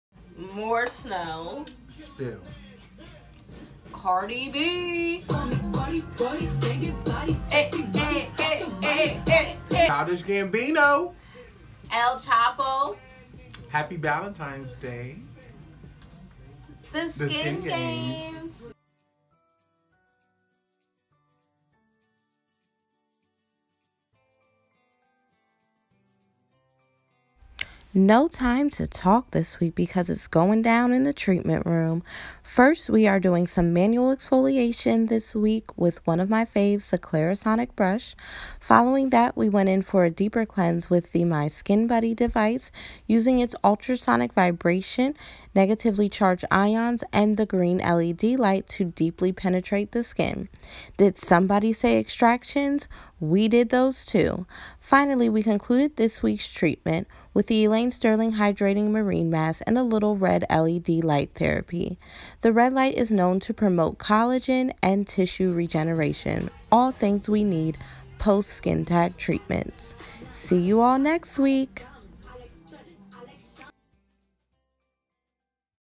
Regilynn Haywood in The New Esthetician Category for The Skin Games 2019 | week 4
Skincare with Regilynn on Valentine's Day is really productive!